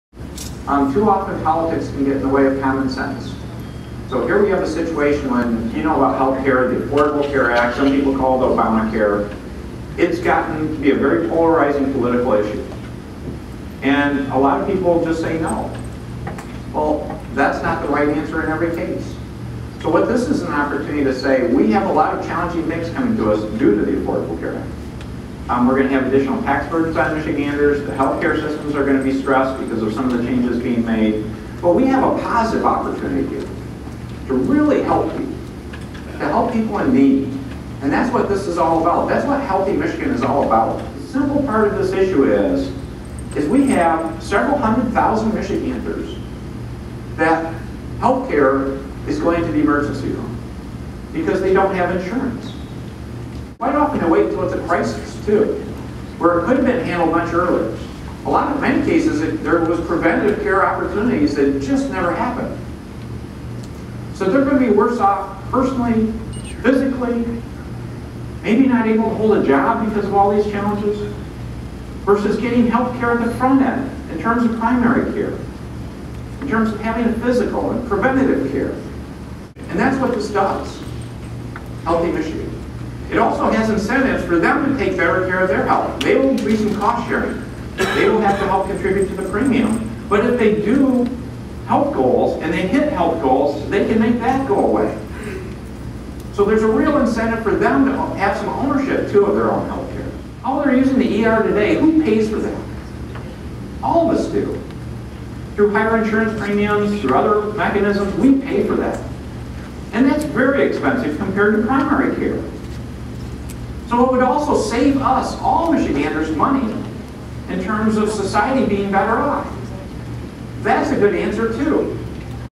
Governor Snyder: Healthy Michigan Is Right for Michigan
Michigan Governor Rick Snyder explains why the Healthy Michigan plan to reform Medicaid and expand coverage to half a million low-income, working Michiganders, is good for the state. He spoke during a town hall at Genesys Health System in Grand Blanc on June 27, 2013.